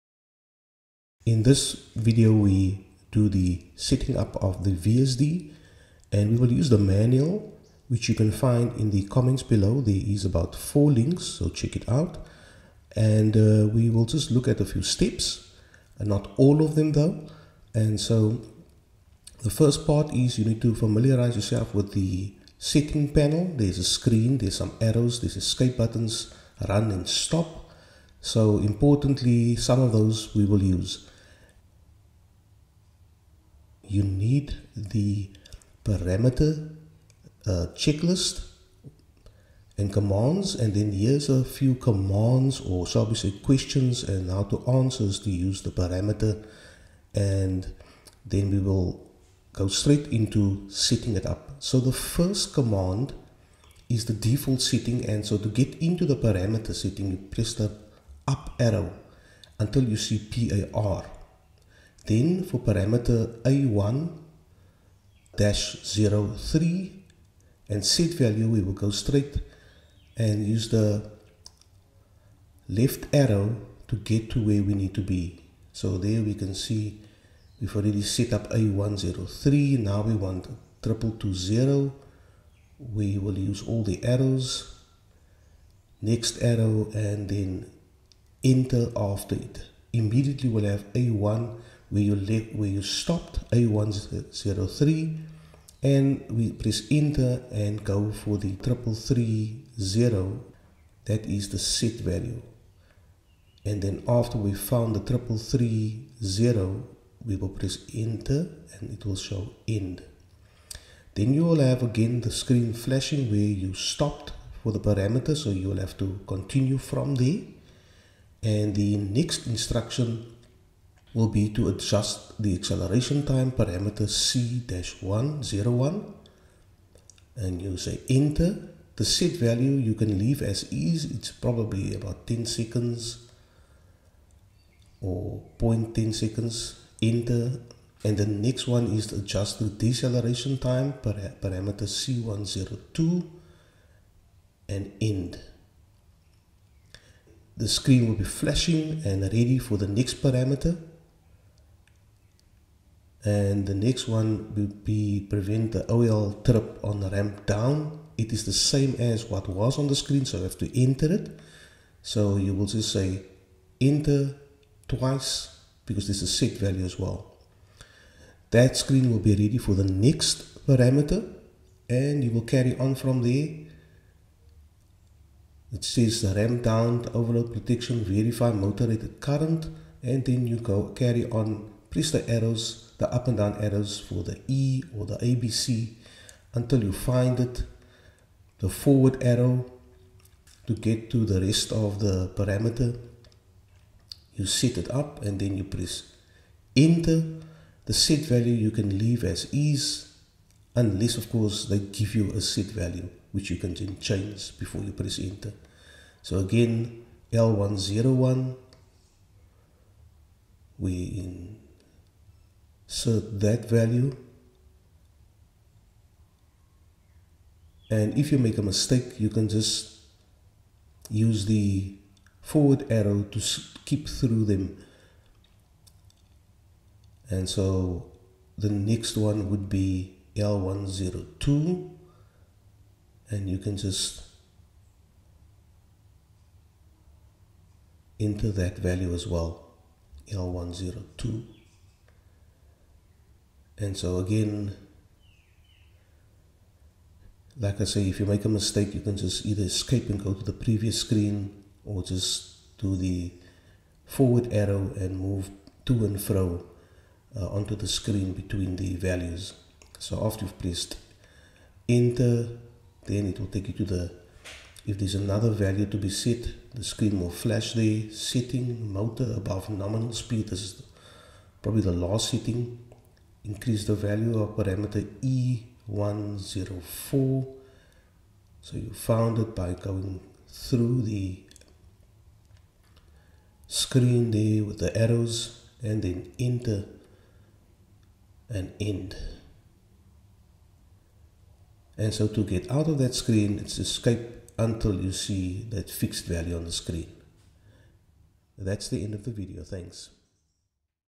VSD QUICK PROGRAMMING
This video is a follow-up from the VSD CONNECTING video and specifically deals with it's programming.